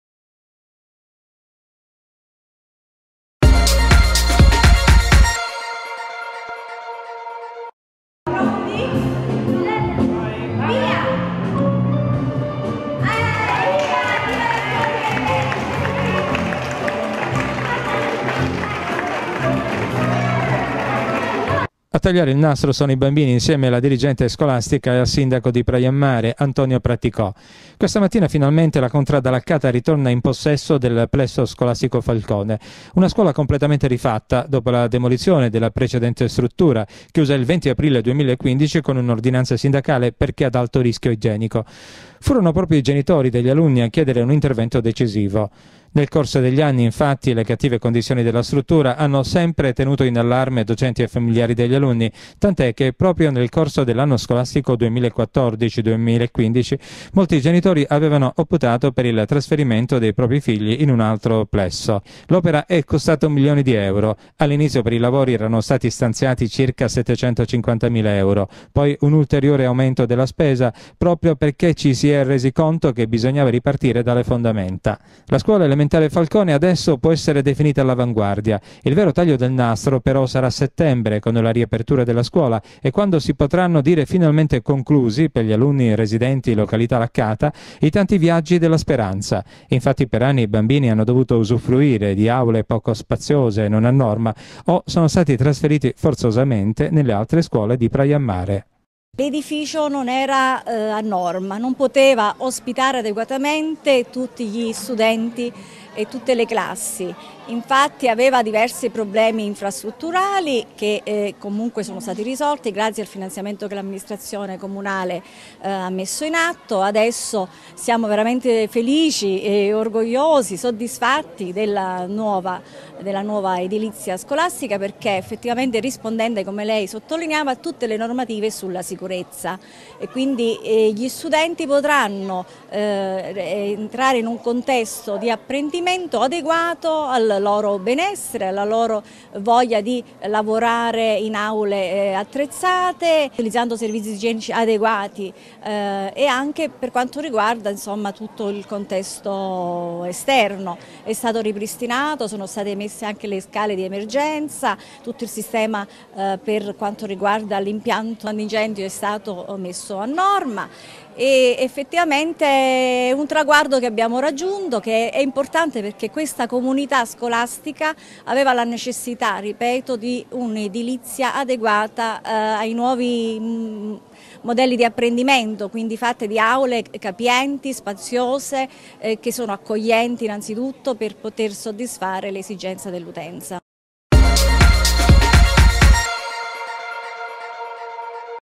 Scuola Falcone, Praiamare (Cs): inaugurato nuovo plesso della Laccata
Finalmente, la contrada Laccata ritorna in possesso del plesso scolastico Falcone.